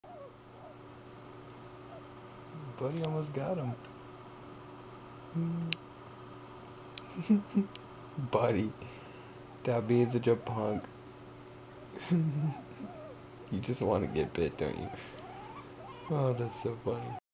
YOUTUBED Pepper bopping Buddy.MOV
My cat, Pepper, being curious.